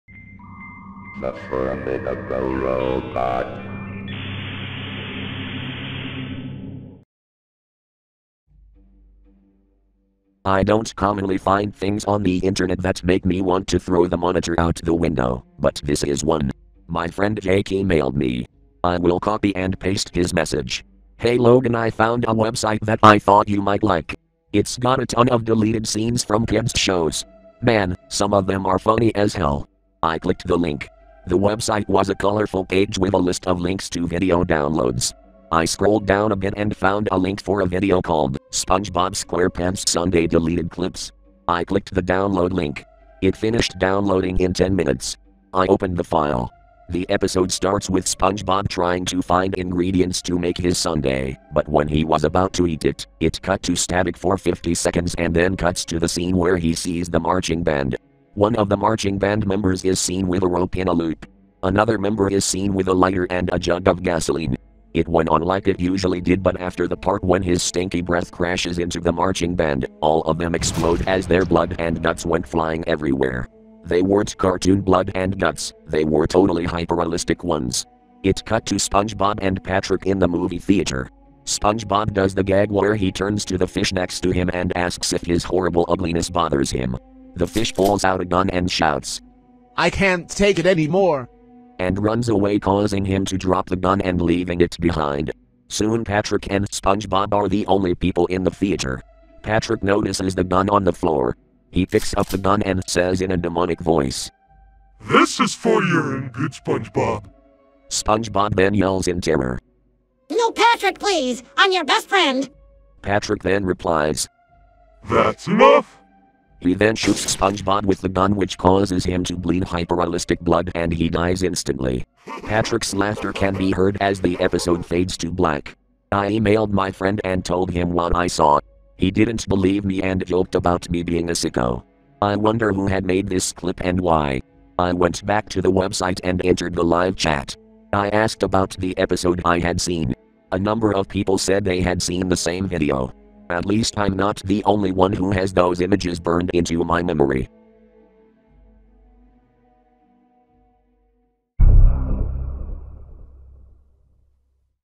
(Creepypasta) SpongeBob Lost Episode: SpongeBob's Sunday
WARNING: Contains really graphic and impulsive storytelling.

Credit: 15.ai, Unknown Author, and Uberduck

Sound FX from:
FindSounds.com
FreeSFX.co.uk
Freesound.org
SoundBible.com
Sounddogs.com
The-Sounds-Resource.com
YouTube.com